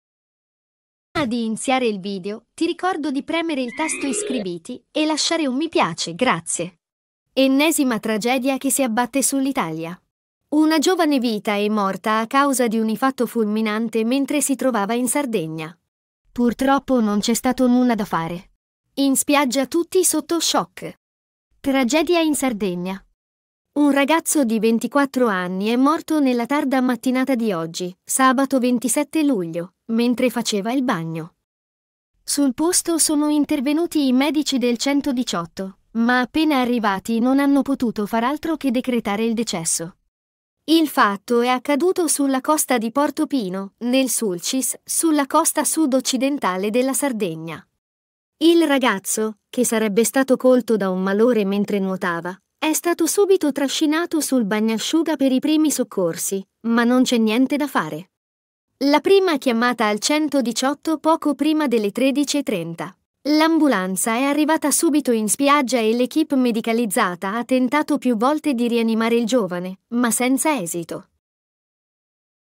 TRAGEDIA IN ITALIA: Giovane ragazzo muore per un tuffo in mare, solo ora si scopre che...
INFARTO FULMINANTE MENTRE FA IL BAGNO AL MARE: muore giovane di 24 anni in sardegna